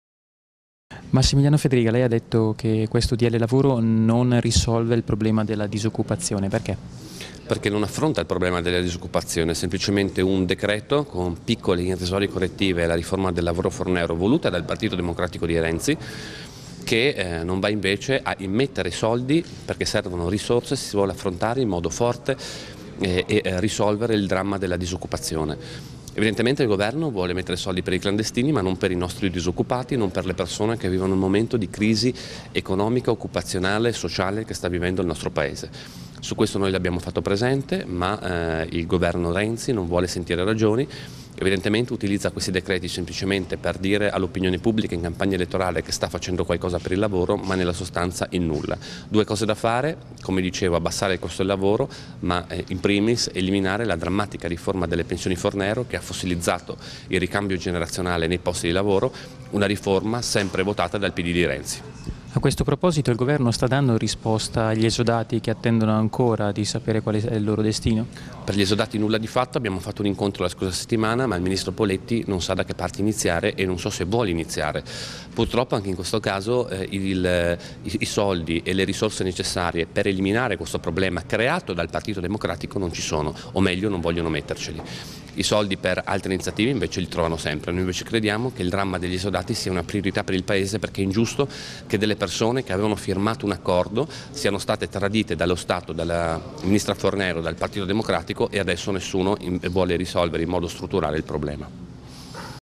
DL LAVORO: FEDRIGA (LN), NON RISOLVE DRAMMA DELLA DISOCCUPAZIONE
ROMA, 12 MAG -- "Il decreto lavoro è un provvedimento inutile che non risolve in alcun modo il dramma della disoccupazione nel Paese ma vuole semplicemente soddisfare un'esigenza elettorale del presidente del Consiglio".

Lo ha detto in Aula il capogruppo della Lega Nord in commissione Lavoro alla Camera, Massimiliano Fedriga.

"L'unica via per rilanciare il mercato occupazionale nel breve periodo è di cancellare la riforma delle pensioni Fornero, che sta colpendo al cuore le imprese e ha generato il dramma degli esodati. Se il governo, come sembra,  metterà la fiducia anche su questo decreto sarà una scelta mirata a coprire le divisioni della maggioranza".